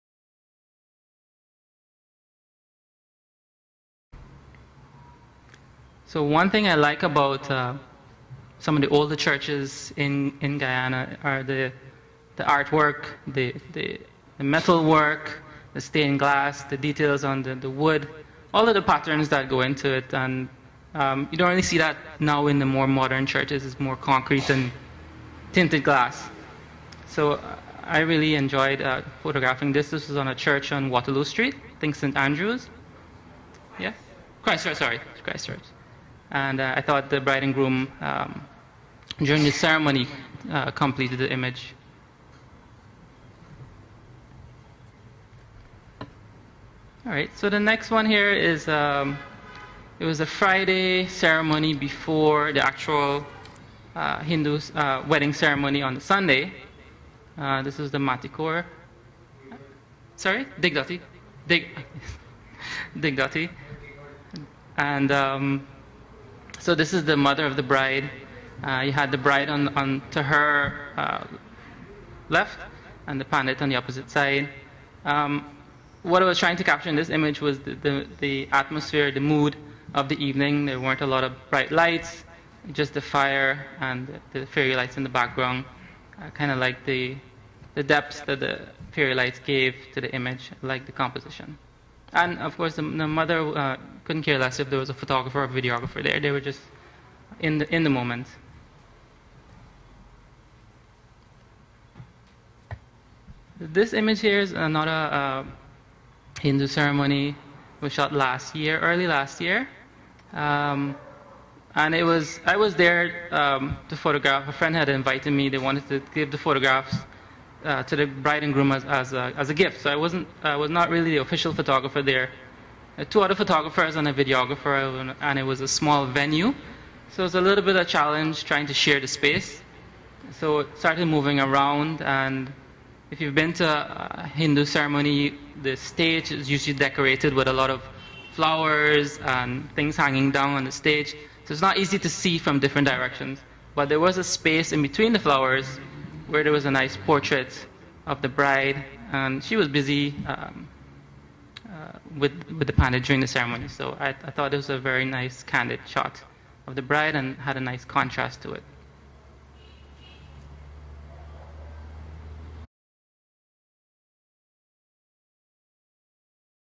Art In Weddings (Fidal Bassier) Part Two
Guyanese photographer Fidal Bassier explores a series of his images in this clip from a presentation at Moray House Trust in September 2014.